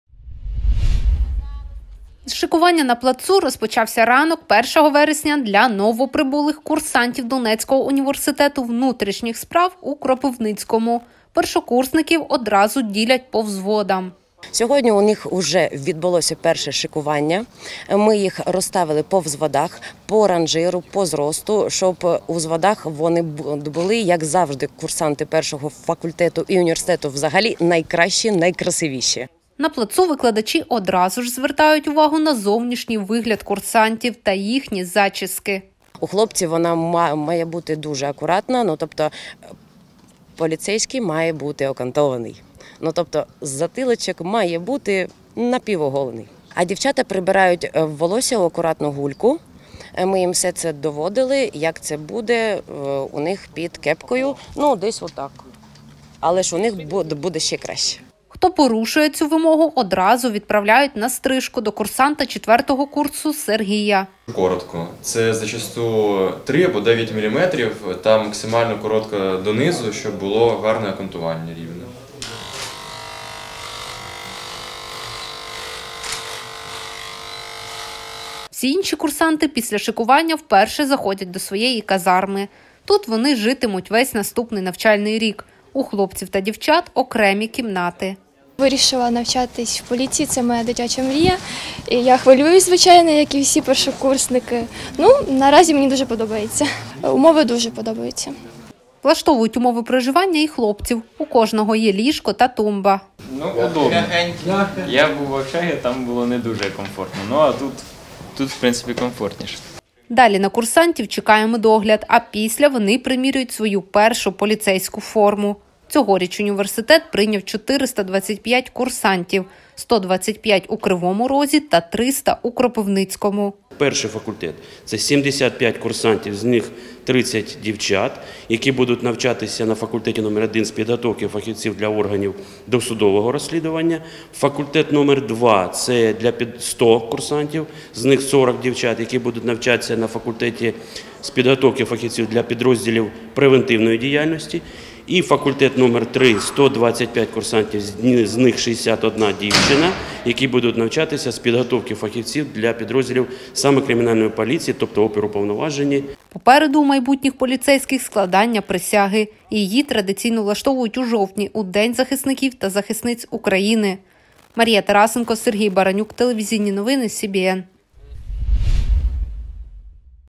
300 курсантів вступили до Донецького університету внутрішніх справ у Кропивницькому |телеканал Вітер
З шикування на плацу розпочався ранок першого вересня для новоприбулих курсантів Донецького університету внутрішніх справ у Кропивницькому. Після перевірки зовнішнього вигляду та зачісок курсанти заходять до своєї казарми. Далі на курсантів чекає медогляд, а після вони приміряють свою першу поліцейську форму. Цьогоріч університет прийняв 425 курсантів – 125 у Кривому Розі та 300 у Кропивницькому.